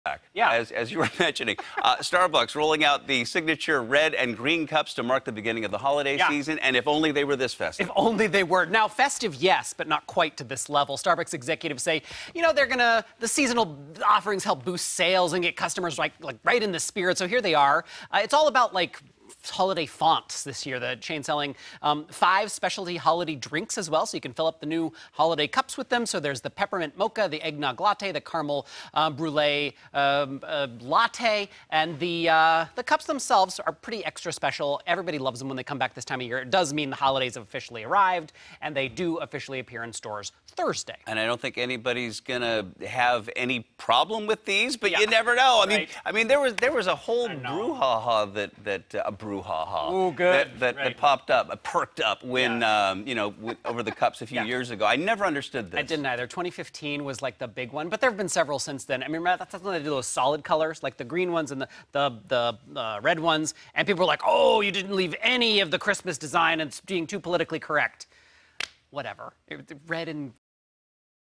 Starbucks’ holiday cups will be back this week
Christmas time has arrived at Starbucks once again.

The coffee chain said on Wednesday that its holiday drinks, foods and its signature red and green cups will be back in stores on Thursday.

For Starbucks, the seasonal offerings are a way to create buzz, give their fans what they want and boost sales.